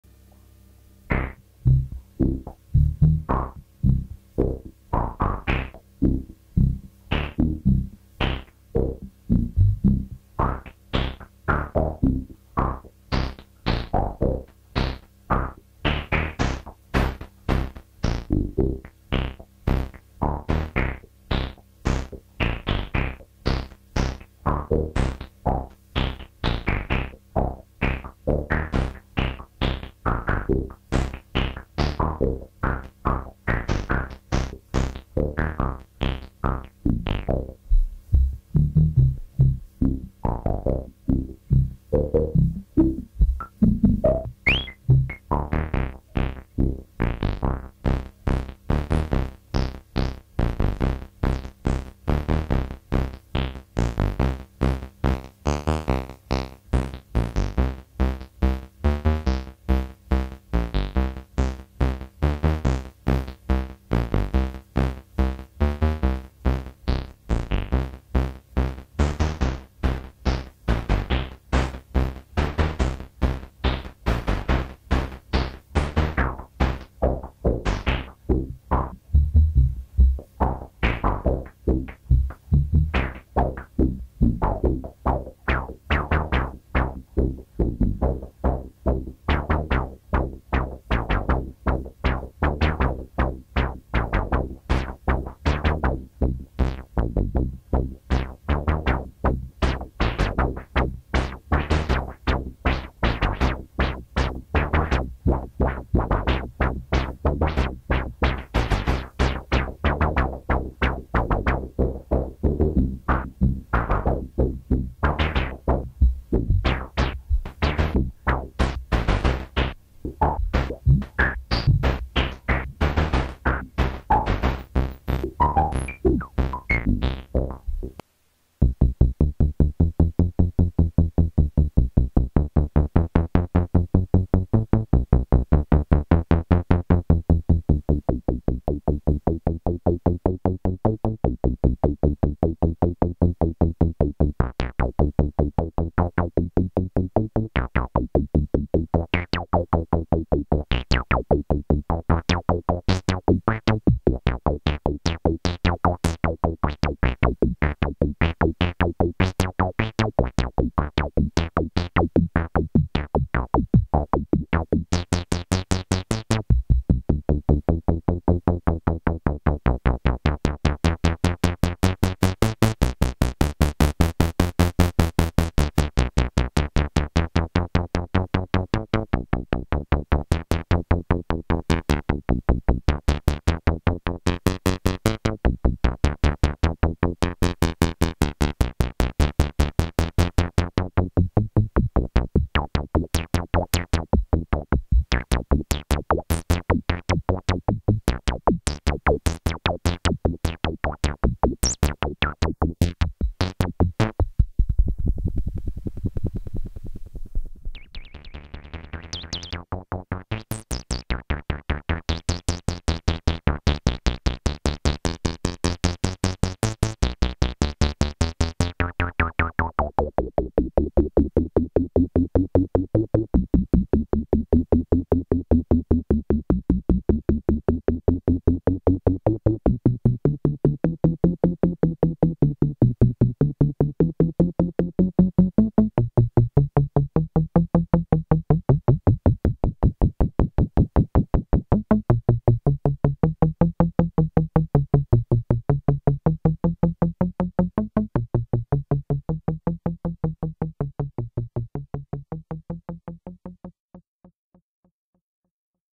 barn1x8
Adam painting panto scenery, cloud 9. Adam on synth .